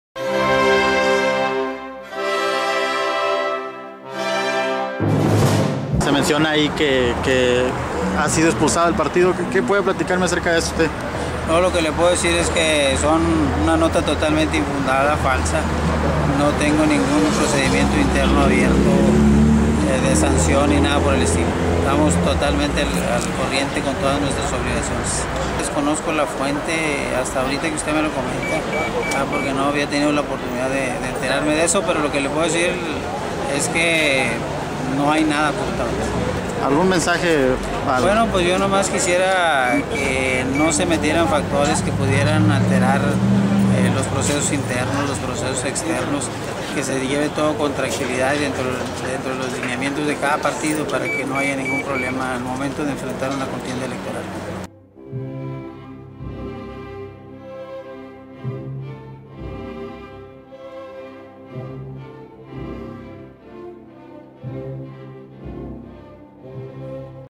Gelacio Marquez Segura, Diputado Local
Desmiente Diputado del PAN expulsión del partido